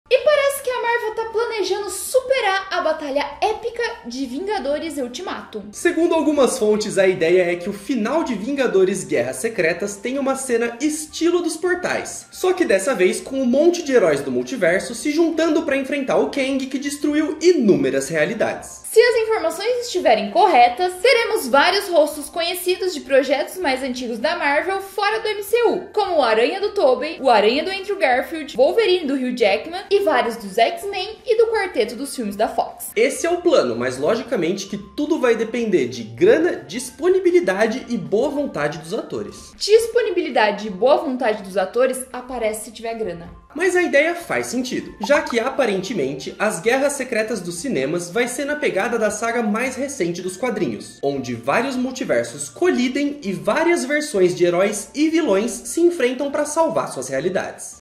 A Batalha ÉPICA de Vingadores Guerras Secretas!
E parece que a batalha vem ai uma batalha épica pra superar Vingadores: ultimato, já que segundo alguns rumores Guerras Secretas vai ter a participação de vários Homem-Aranha, Wolverine, X-Men e Quarteto-Fantástico dos filmes antigos da Marvel!